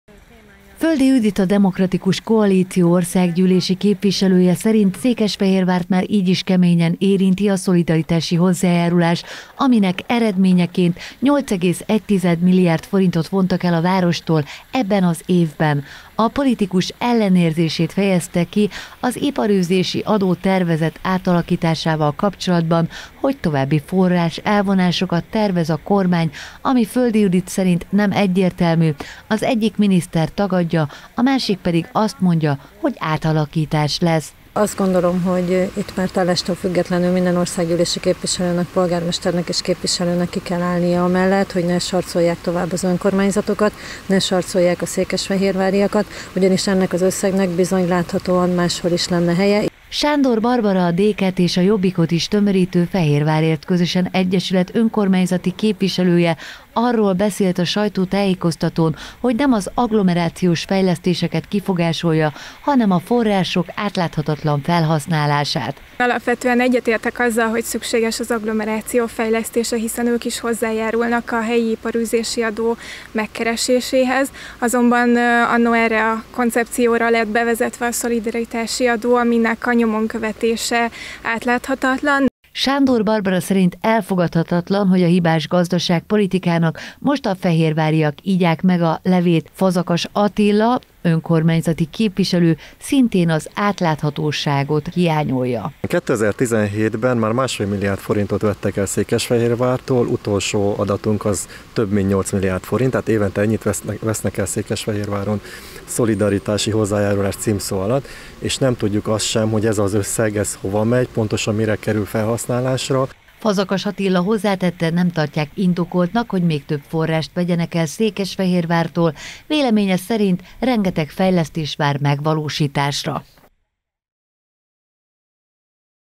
Ellenzék a helyi iparűzési adó tervezett átalakításáról
A helyi iparűzési adó tervezett átalakítása kapcsán tartott sajtótájékoztatót Földi Judit a DK országgyűlési képviselője, valamint Sándor Barbara és Fazakas Attila a Fehérvárért Közösen Egyesület önkormányzati képviselői.